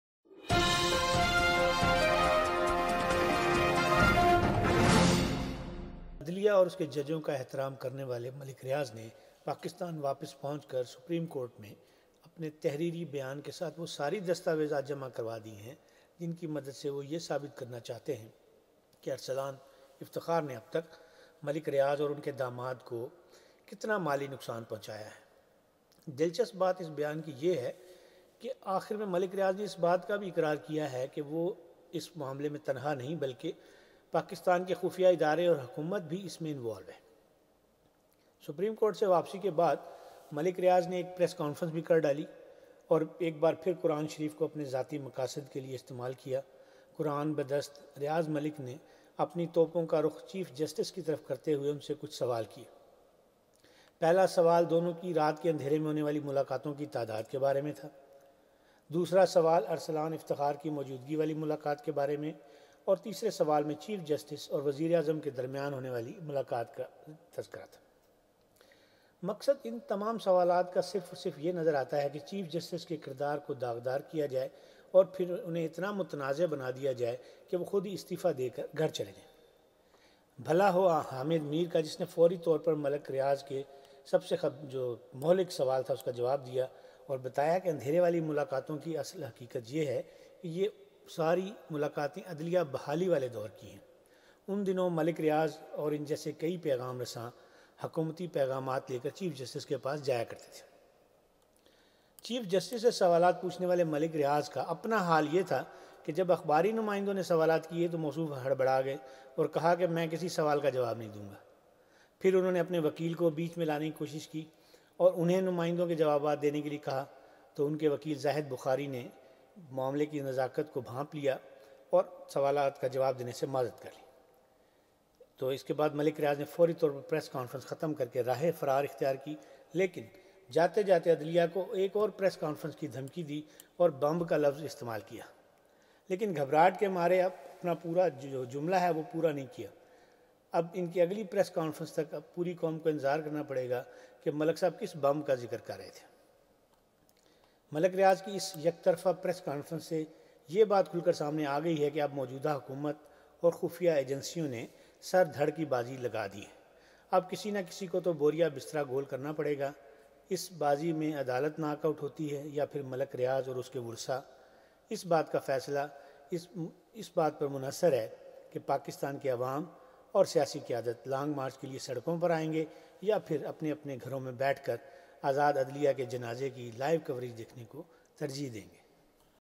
Malik Riaz Press Conference 12 June 2012 Blaming Chief Justice Iftikhar Chaudhry
Bahria Town chief Malik Riaz  while holding Quran Sharif claimed in his press conference that he met Chief Justice Iftikhar Chaudhry in the presence of his son, Arsalan Iftikhar. He said that the Chief Justice knew about the business dealings and should have taken suo moto action before the media broke this news.
Khrian Khrian 12/06/2012 by Rashid Murad...RM TV London